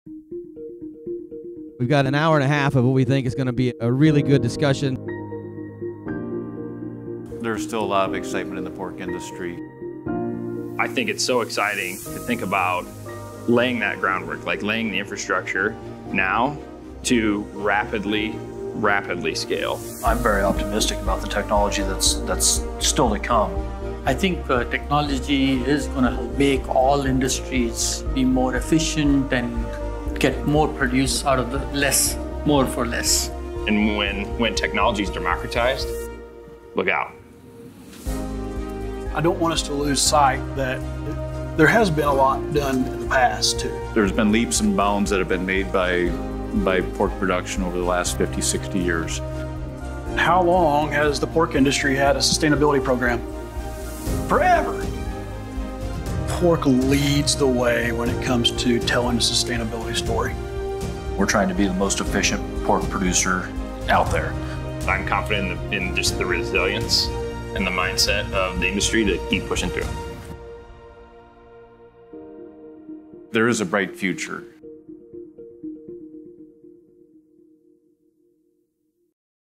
World Pork Expo Panel Trailer
Mark Lyons, President and CEO of Alltech, along with panelists from the pork industry discussed the role of digital technology in animal agriculture and strategies for creating environmental and economic sustainability at the 2023 World Pork Expo.